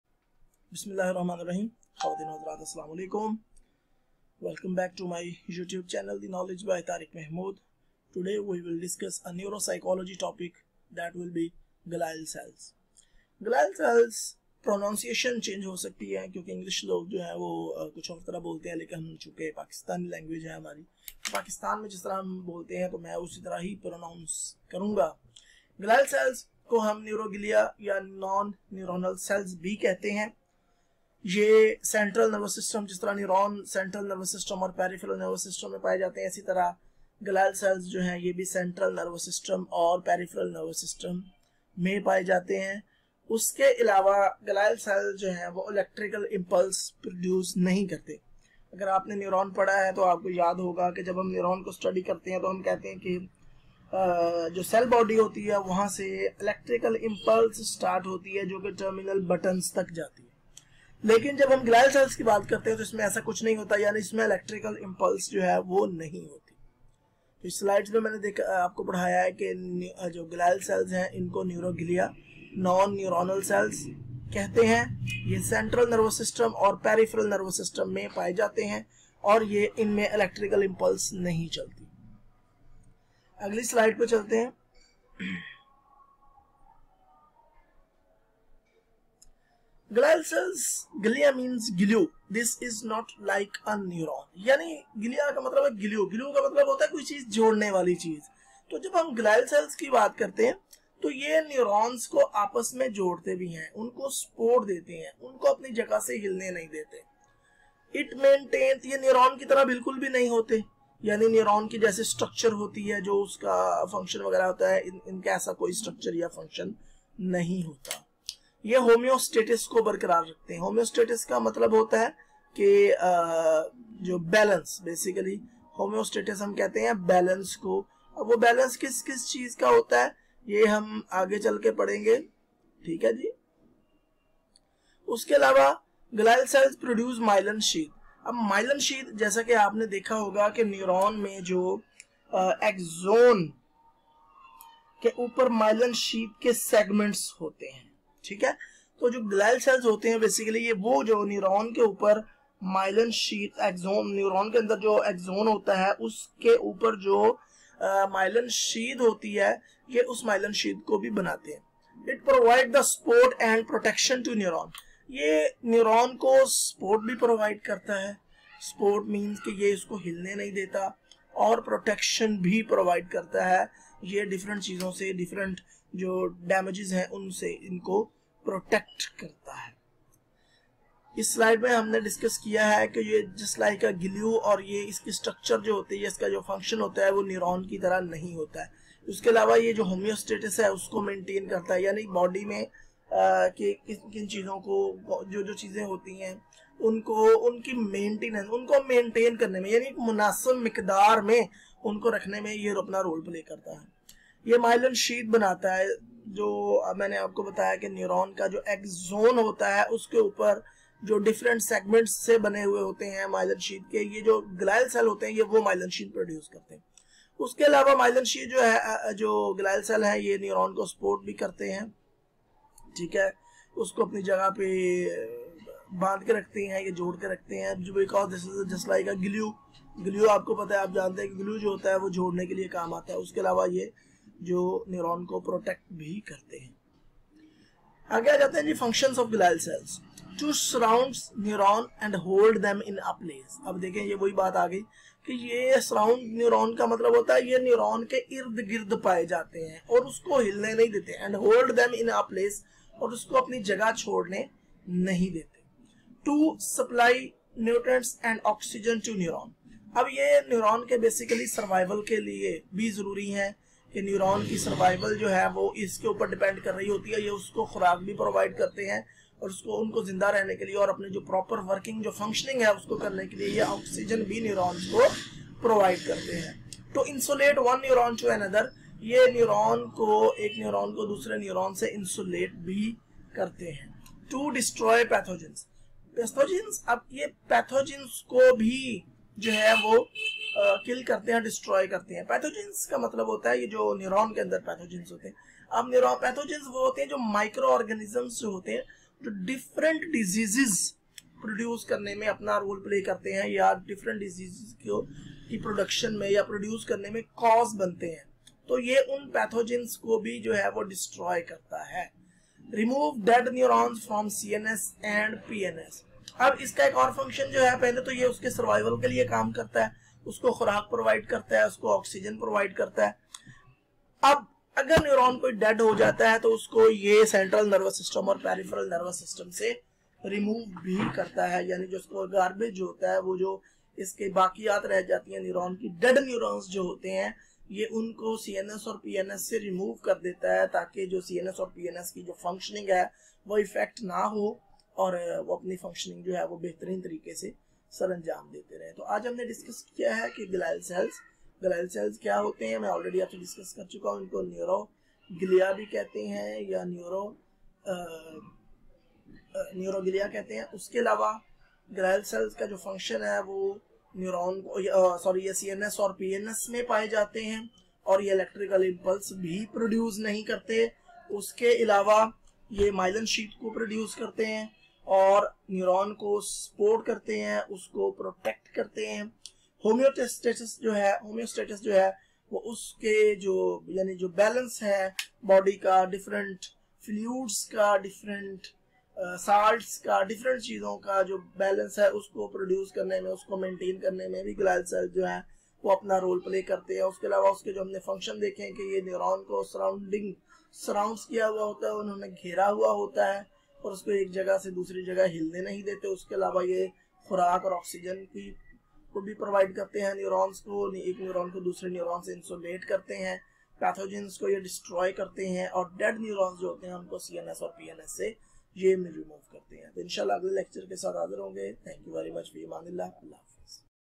Glial Cells ! Neuroglia ! Non neuronal Cells / Neuropsychology
Glial cells are protective sheaths for neurons. it provides the support, nutrients, and oxygen to the neurons.